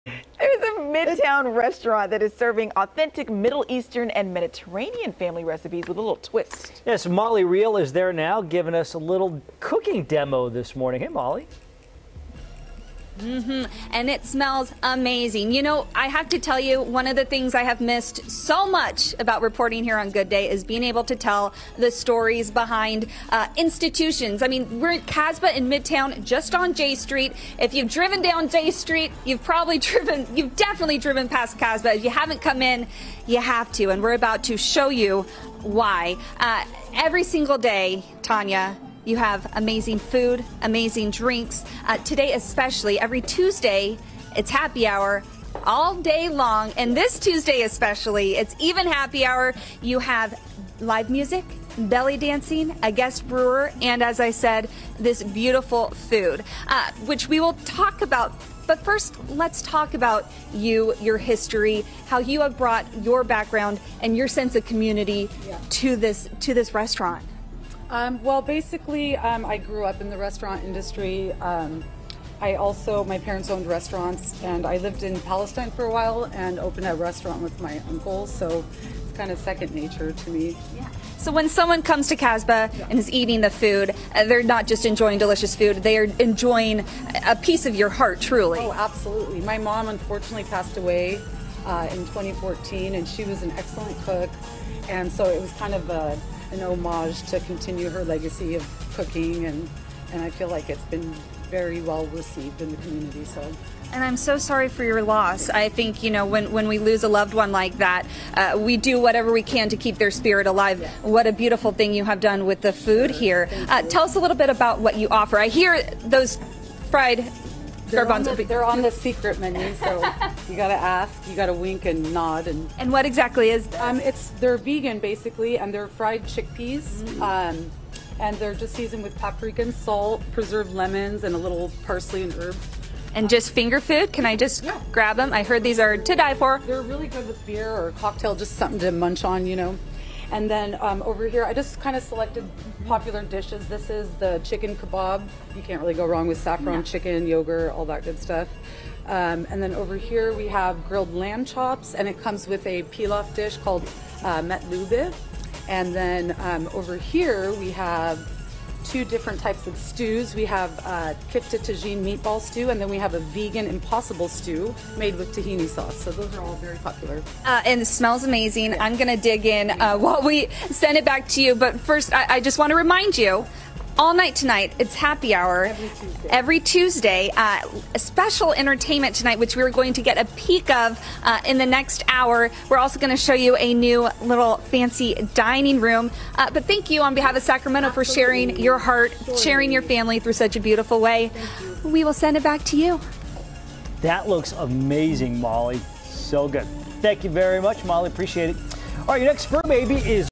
Kasbah
Kasbah, located in the heart of midtown Sacramento on J Street, is an authentic Middle Eastern and Mediterranean restaurant and bar – serving hand crafted cocktails, and family recipes with a modern twist.